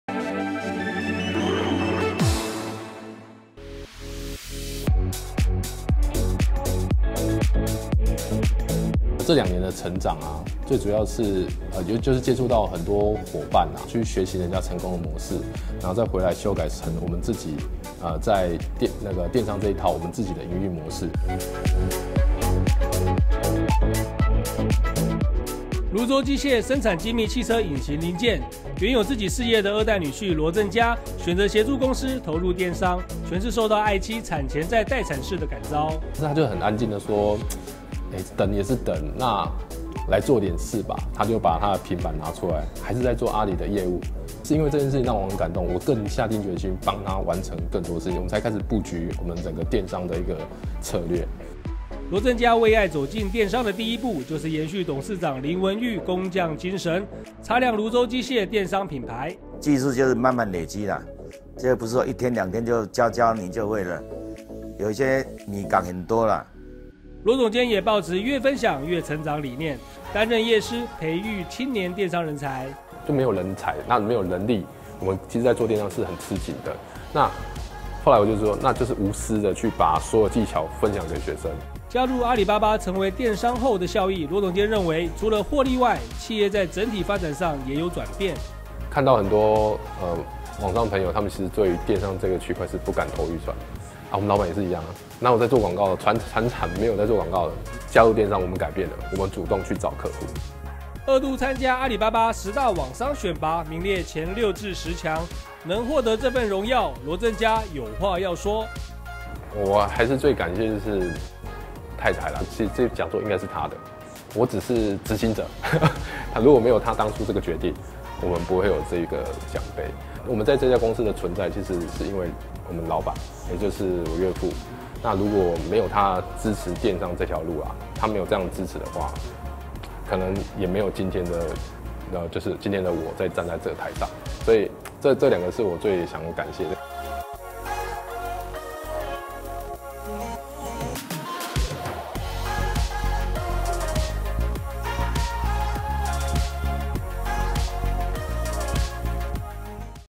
延續傳產工匠技藝 蘆洲機械羅振嘉為愛進電商【阿里巴巴十大網商】
蘆洲機械生產精密汽車引擎汽門零件，以高精密、高技術獲客戶肯定。原有自己事業的二代女婿羅振嘉，受到愛妻產前在待產室的感召，決定協助公司投入電商，並獲2019阿里巴巴十大網商選拔前六至十強。待產室裡究竟發生什麼事？如何進行電商布局？來看行銷總監羅振嘉的故事。 (11TV 記者/陳恆光 攝影/張至友 剪輯/林新造)

想要有員工聚餐、下午茶、慶生會、員工旅遊等#吃喝玩樂 的幸福企業 點這裡